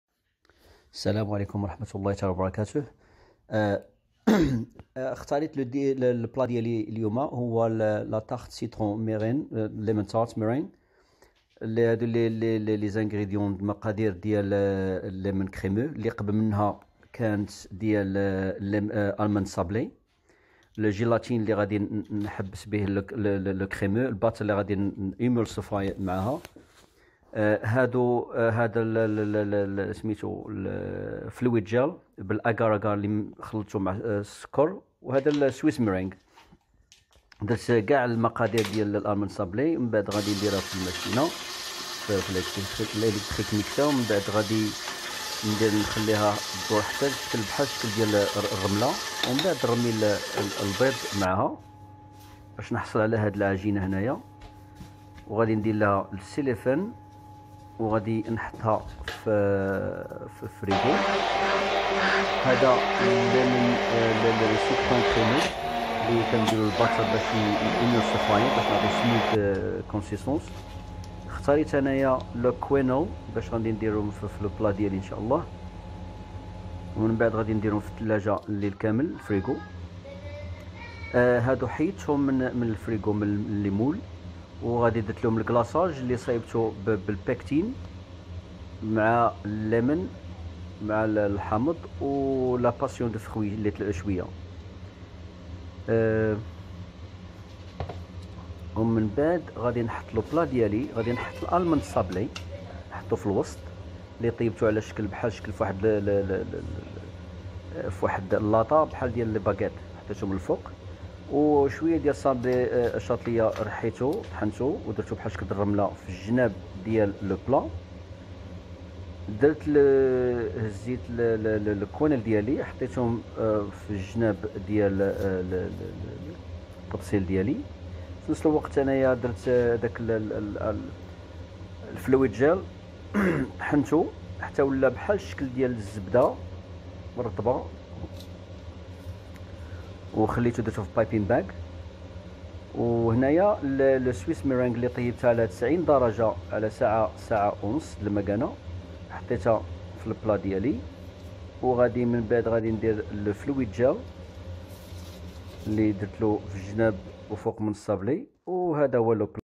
Tart au citron de (Mustapha Rahimi)
Vidéo de اللجنة المغربية للفنون الطهي (المسابقة الوطنية

TART AU CITRON “P” SP00782
SABLE D’AMANDE
360g de farine nature
140 g de sucre glace
50 Amande moulue
Pincée de sel
180 g de beurre non salé
2 Oeufs
CRÉMEUX CITRON
80g de jus de citron
30 Fruit de la passion
2 Zeste de citron
100g Oeufs
50g Jaunes d’œufs
120 g de sucre en poudre
2 g de feuilles de gélatine
80 g de beurre non salé
MERINGUE SUISSE
150g Blancs d’œufs
300 g de sucre en poudre
GELS LIQUIDES D’AGRUMES/ GELÉE
200g de jus d’orange
50 g de fruit de la passion
20g de jus de citron
5 g de sucre en poudre
2 g d’agar-agar
GLAÇURE AU CITRON
8 g de Pectin
100 g de sucre en poudre
300 g de purée de citron
50 g de purée de citron
130g d’eau
110g de glucose